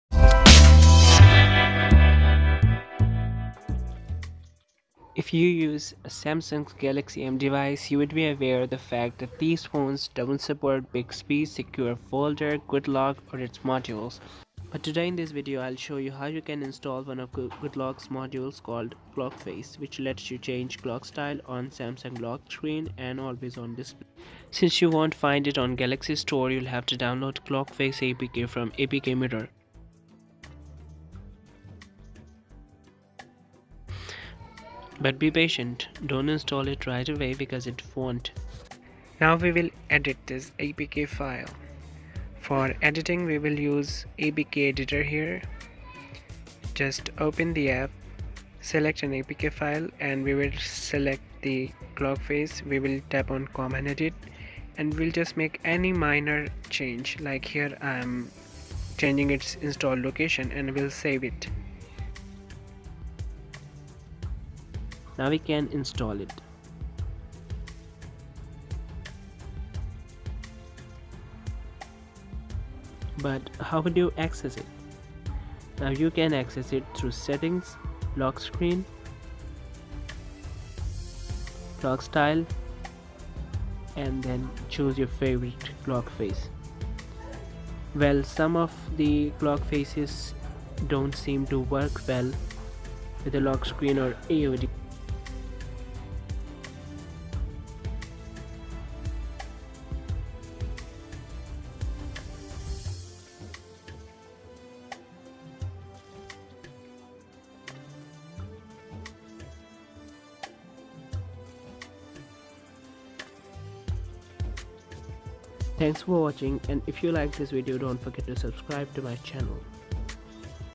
How to Install Samsung Clockface on Galaxy M30/M30s/M20/M10/M40
Galaxy M series phones don't support Goodlock or it's modules. But watch this video to know how you can install Clockface- a goodlock module on Galaxy M series phones.